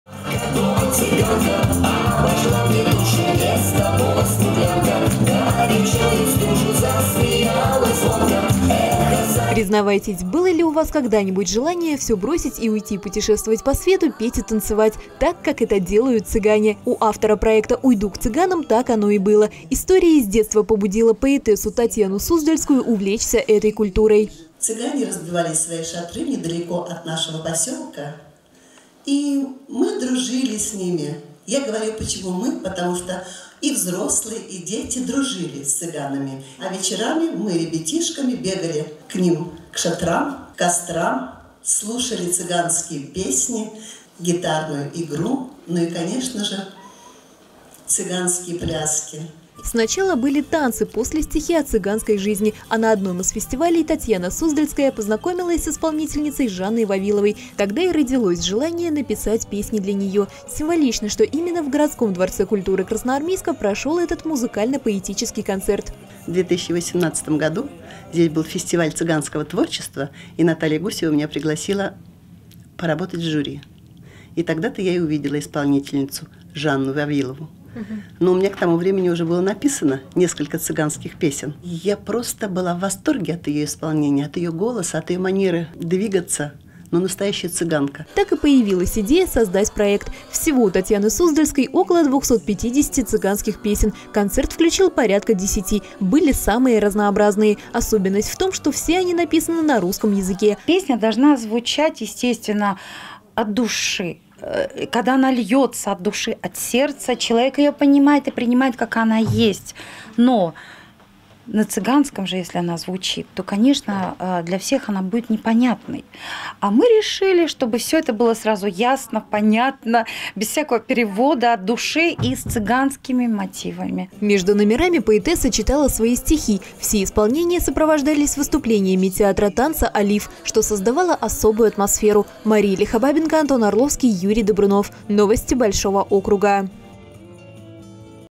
"Уйду к цыганам!" Авторский концерт цыганских песен прошёл в Красноармейске
Жизнь цыганская вольна! Яркие наряды, манящие танцы и душевные песни. Прикоснуться к этой культуре смогли жители нашего округа. В Красноармейске прошёл авторский концерт цыганских песен.